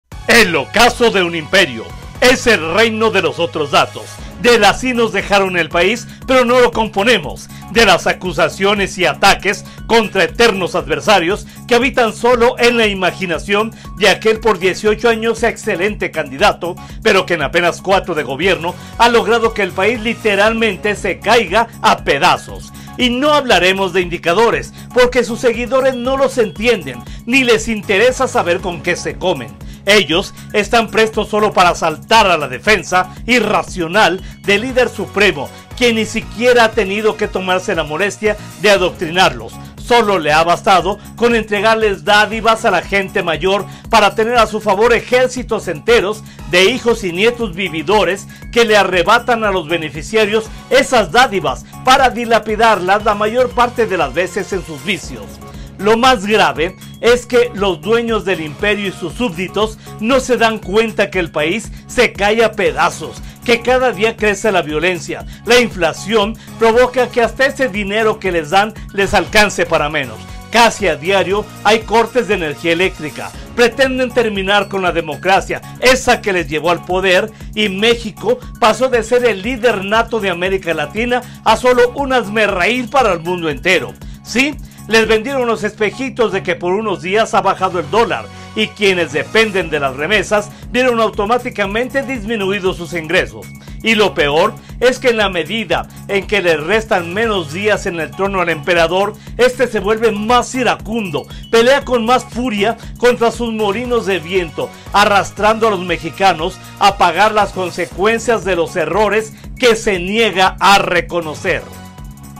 Crónica del Poder/El ocaso de un imperio
Aguascalientes, Ags. 31 enero 2023.- Es el reino de los otros datos, del así nos dejaron el país, de las acusaciones y ataques contra los eternos adversarios que habitan solo en la imaginación de aquel por 18 años muy excelente candidato, pero que en apenas 4 de gobierno ha logrado que el país, literalmente, esté cayéndose a pedazos.
Y no hablaremos de indicadores, porque sus seguidores simple y sencillamente no los entienden, ni les interesa saber siquiera con qué se comen. Ellos están prestos solo para saltar a la defensa irracional del líder supremo, quien ni siquiera ha tenido que tomarse la molestia de adoctrinarlos, sólo le ha bastado con entregarles dádivas a la gente mayor, para tener a su favor ejércitos enteros de hijos y nietos vividores que le arrebatan a los beneficiarios esas dádivas para dilapidarlas la mayor parte de las veces en vicios.
Lo más grave es que los dueños del imperio y sus súbditos no se dan cuenta que el país está cayéndose a pedazos, que cada día crece la violencia, la inflación provoca que hasta las dádivas oficiales alcancen menos, casi a diario hay cortes de energía eléctrica, pretenden terminar con la democracia, esa que les llevó al poder y México pasó de ser el líder nato de América Latina a un hazmerreir para el mundo entero.
Si, les vendieron los espejitos de que por unos días ha bajado el precio del dólar y quienes dependen de las remesas vieron automáticamente disminuidos sus ingresos.
Y lo peor es que en la medida en que le restan menos días en el trono al emperador, éste se vuelve más iracundo, pelea con más furia contra sus molinos de viento, arrastrando a los mexicanos a pagar las consecuencias de los errores que se niega a reconocer.